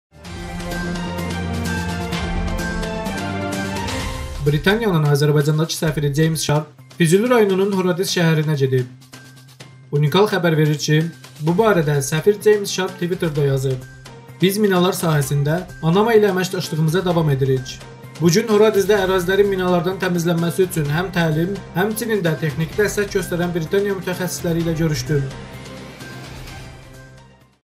Britaniya Səfiri Füzuliyə Səfər Etdi
Britaniyanın Azərbaycandakı səfiri Ceyms Şarp Füzuli rayonunun Horadiz şəhərinə gedib.

"Unikal" xəbər verir ki, bu barədə səfir Ceyms Şarp tviterdə yazıb.

"Biz minalar sahəsində ANAMA ilə əməkdaşlığımıza davam edirik. Bu gün Horadizdə ərazilərin minalardan təmizlənməsi üçün həm təlim, həmçinin də texniki dəstək göstərən Britaniya mütəxəssisləri ilə görüşdüm".